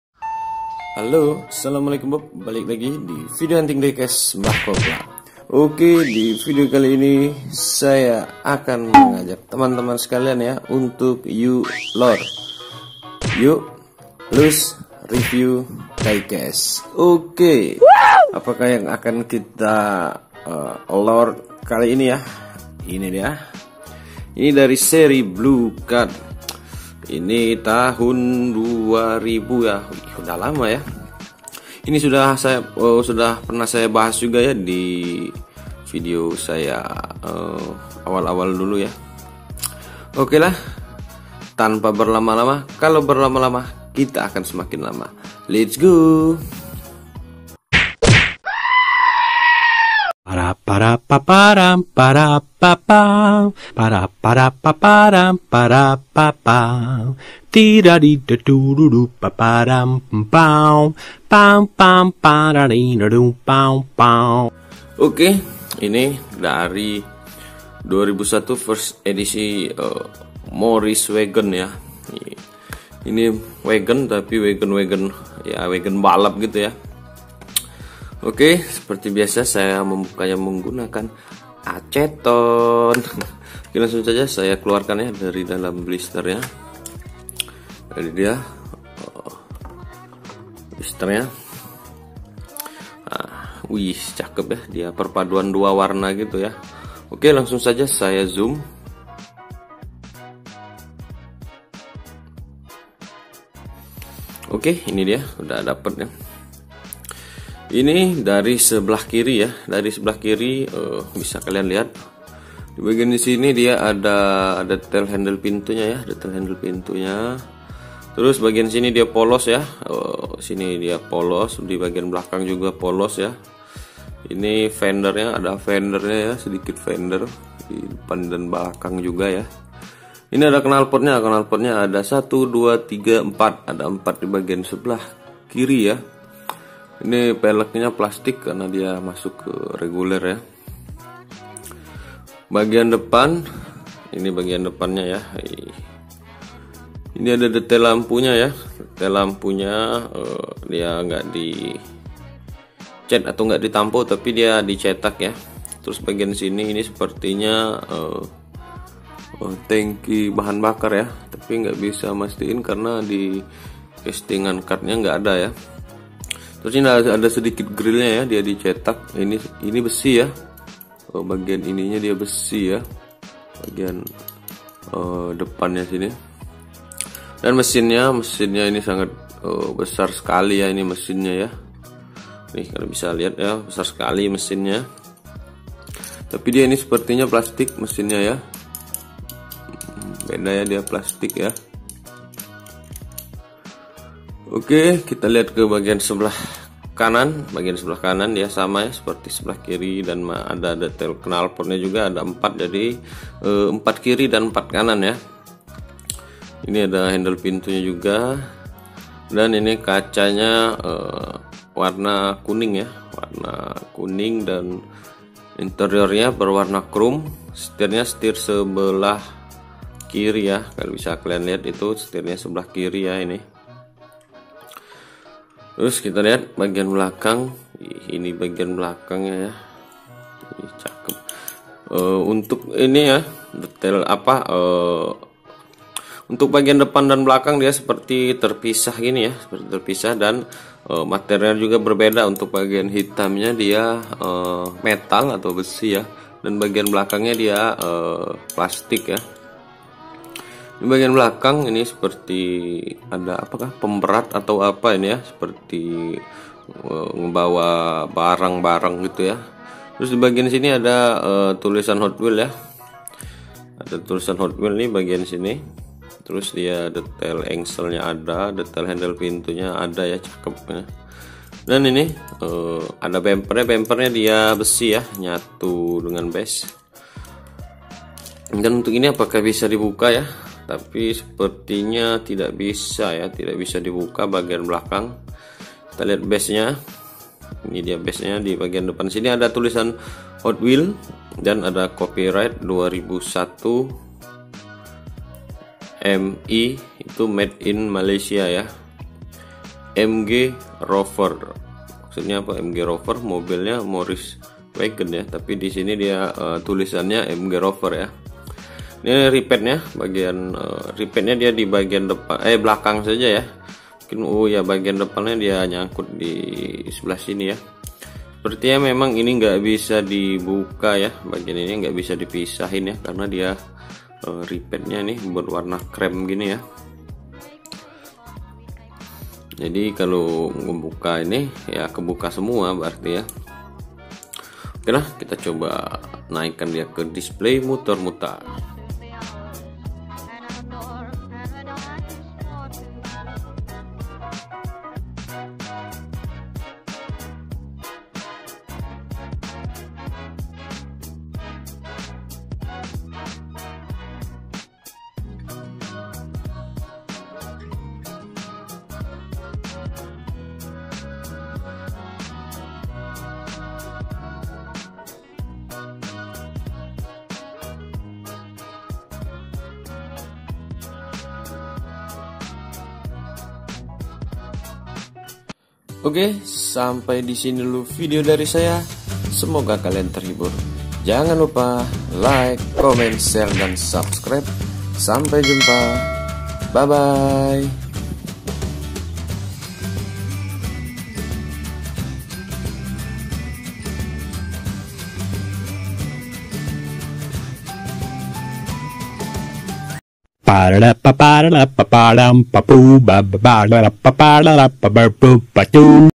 Hunting Diecast l Dobrak Hot Wheels Blue Card First Editions l Morris Wagon l Woo ‼️
Hunting Berbagai Macam Diecast

Dobrak Hot Wheels Blue Card First Editions Morris Wagon

● Saya ucapkan banyak terimakasih kepada kalian yang sudah mampir dan melihat channel serta menonton Video-Video youtube saya